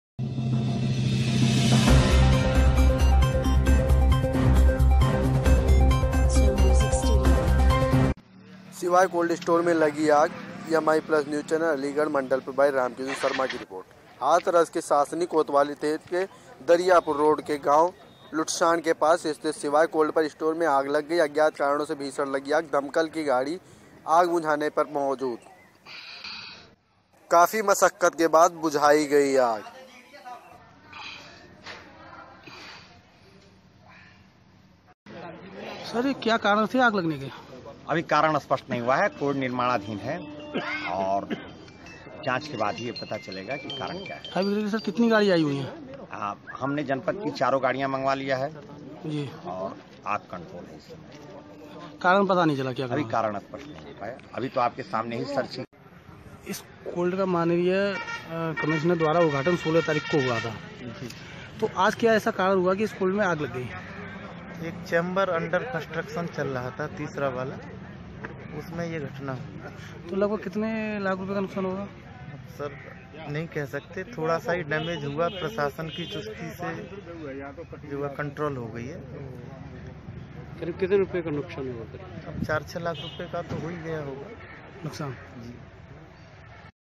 हाथरस की सासनी कोतवाली क्षेत्र के दरियापुर लूट शान गांव के पास सिवाय कोल्ड स्टोर में आग लग गई
Video from miplusnews